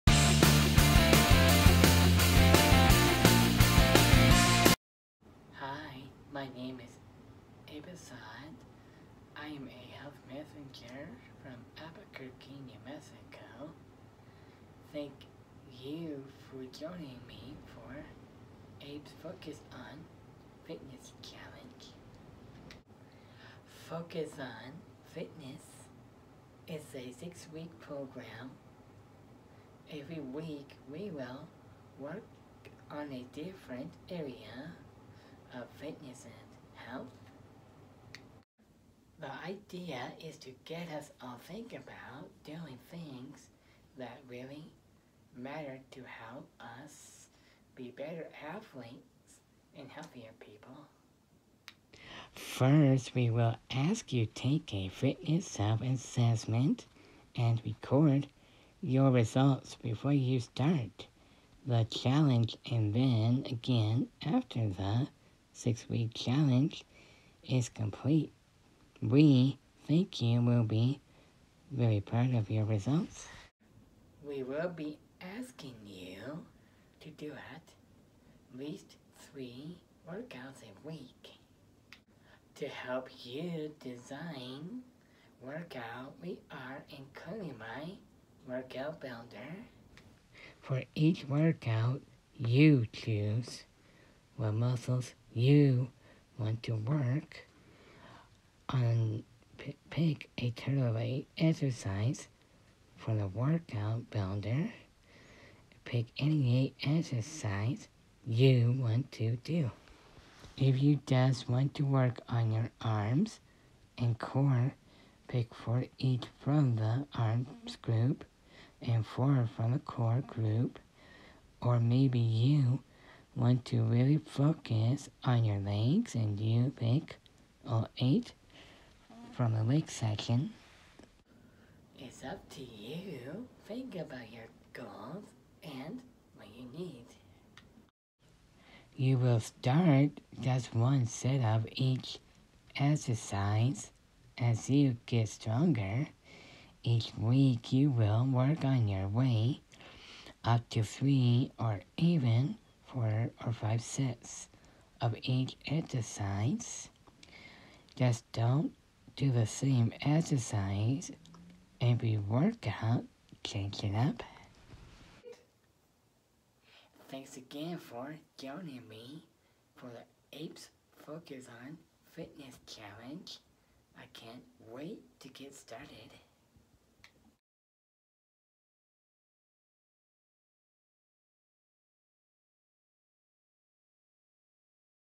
Abe's Focus on Fitness Challenge: Intro
Abe's Focus on Fitness Challenge begins Monday, April 19, 2021. We welcome all Special Olympics athletes to participate in this challenge.

During this 6-week challenge we will:
-Learn new exercises
-Learn how to build a workout
-Build strength that will improve sports performance
-Improve our fitness in small steps
-Learn how to improve our workouts by focusing on form and working out harder